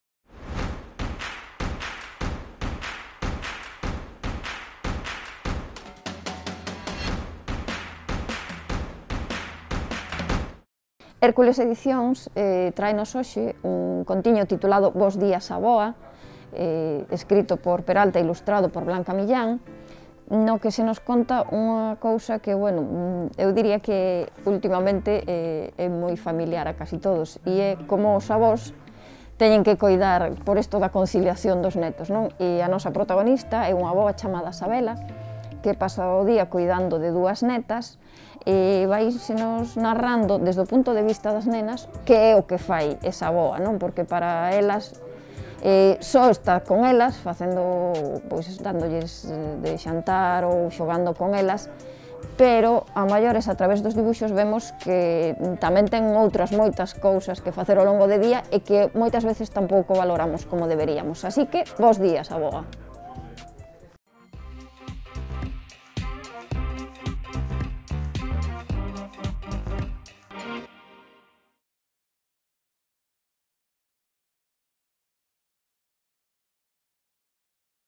Libraría Couceiro: Bos días, avoa
Saben os nenos e nenas o esforzo que fan os avós cando teñen que quedar ao seu coidado? Neste libro de R.J. Peralta e ilustrado por Blanca Millán, 'Bos días avoa' vemos o día a día da avoa Sabela no coidado das súas netas e como, mentres elas pensan que só xoga con elas, a avoa se ocupa de moitas outras cousas.